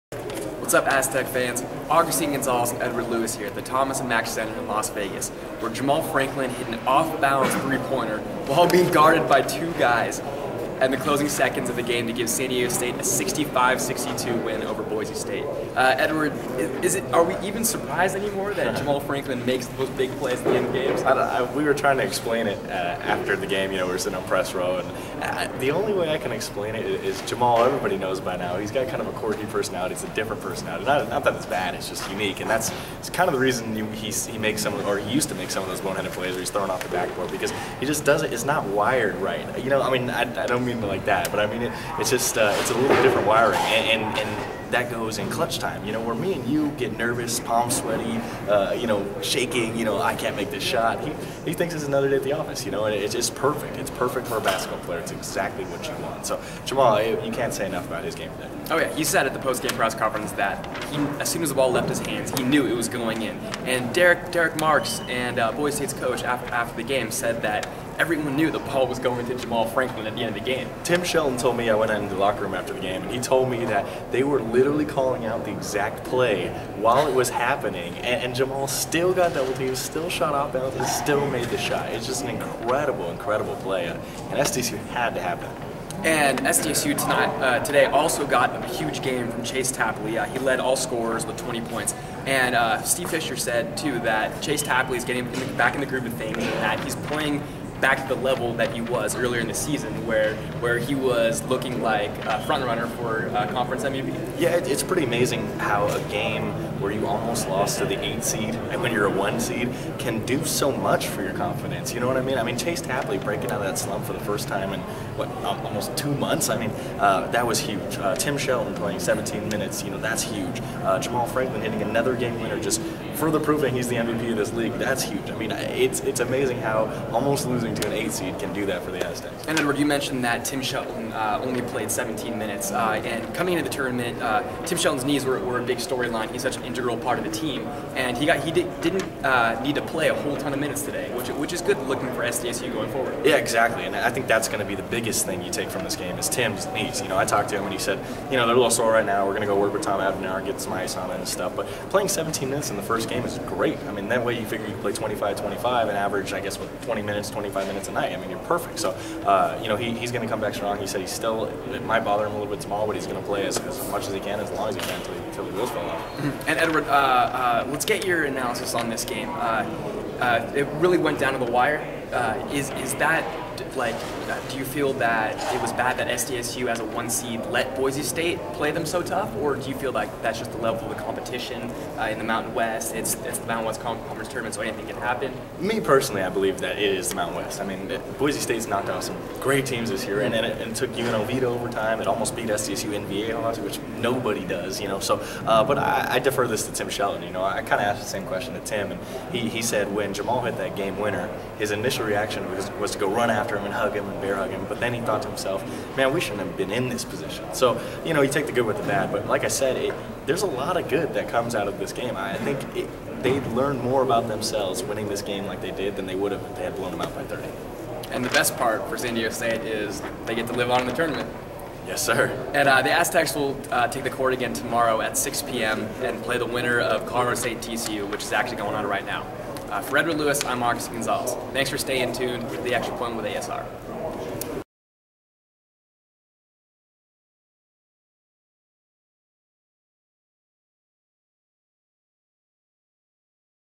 Extra Point with ASR, BSU Recap
AztecSportsReport.com writers Edward Lewis and Agustin Gonzalez break down SDSU's thrilling victory over Boise State on Thursday afternoon.